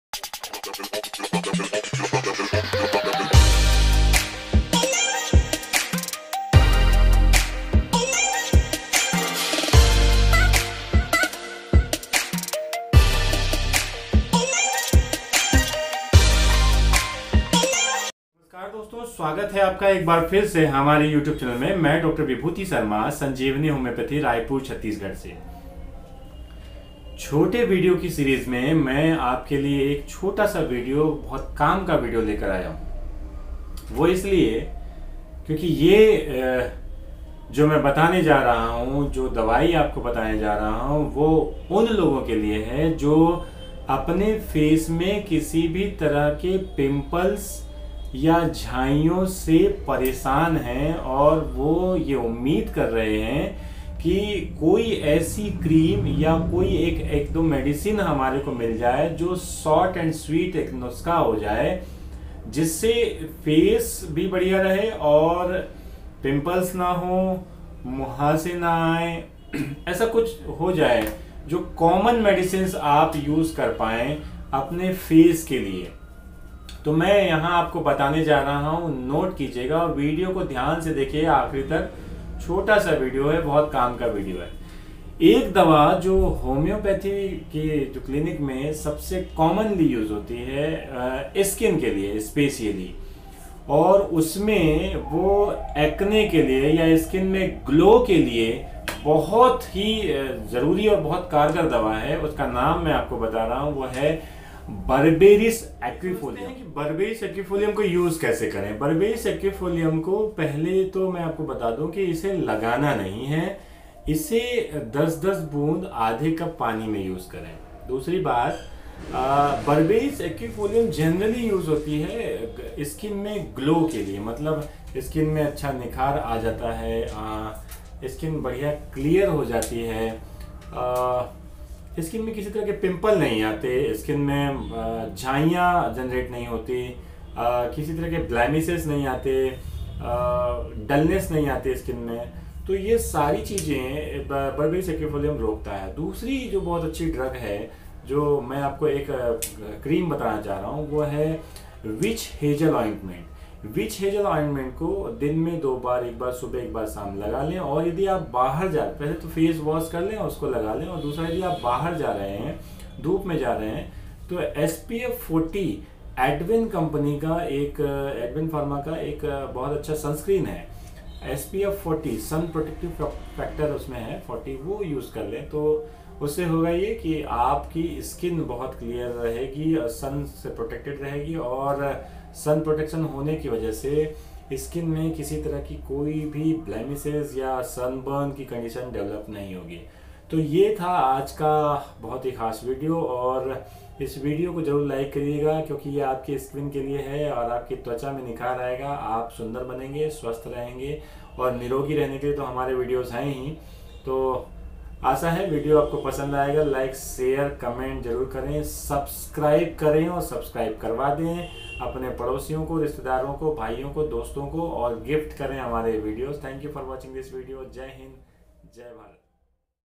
झाइयांFACE Melasma,Hyper pigmentation,झांँईDark CIRCLE BEST HOMEOPATHY MEDICINES Doctor Raipur CG
Medicine: -
1-Berberis Aquifolium Q, 10-10drops
(aadha cup pani me daal kr pina hai)

2-Which Hazel Ointment(2baar lgayein)
3-SPF-40 Adven Pharma
(Sun Protection Sunscreen)